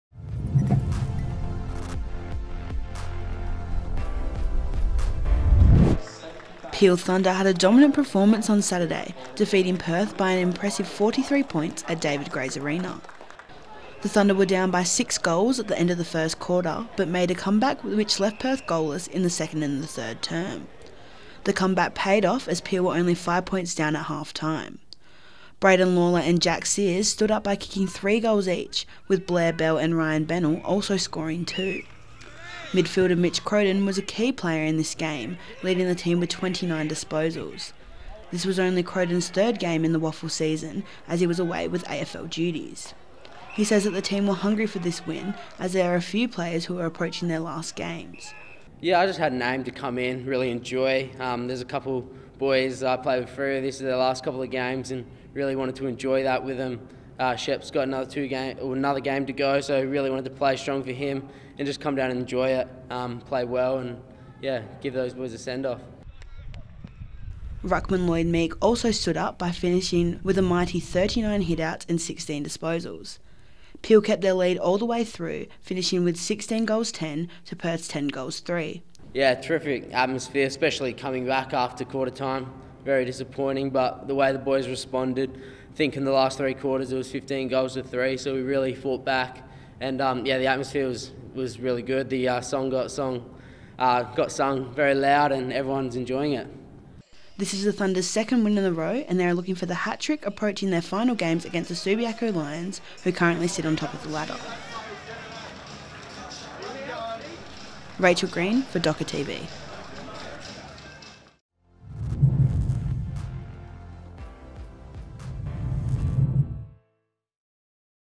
WAFL: Peel's come from behind victory
Peel Thunder pulled off a second straight come-from-behind victory knocking off Perth by 43 points at David Grays Arena on the weekend.